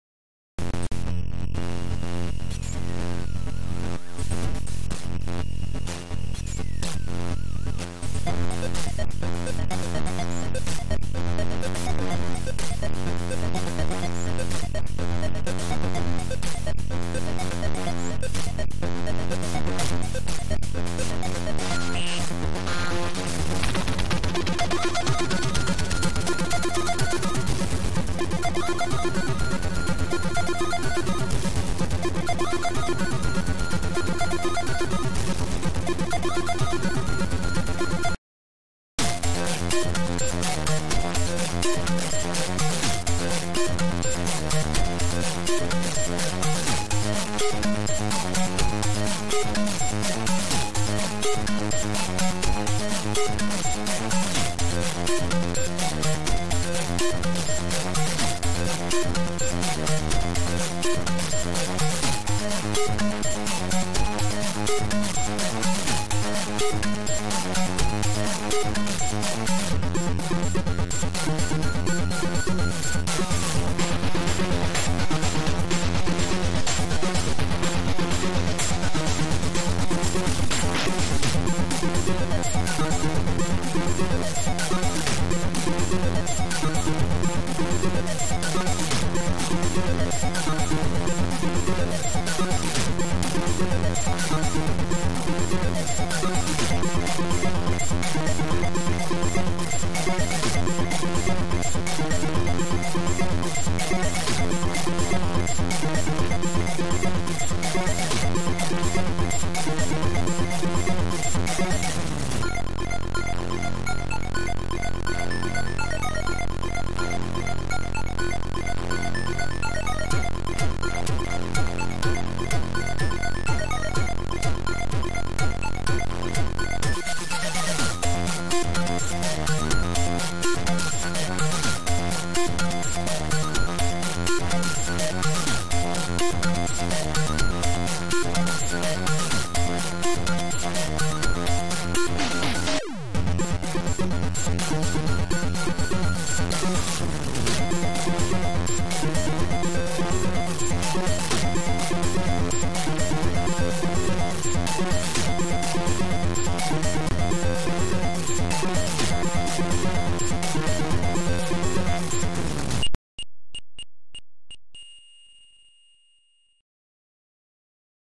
ZX Spectrum 128k: "The End" Demo (2019)
This demo can run on a standard 128k machine. It is made by The Super and you will need Beta Disk 128 or DivIDE / DivMMC with esxdos (The program SAMdisk can change .SCL to .TRD) to run it.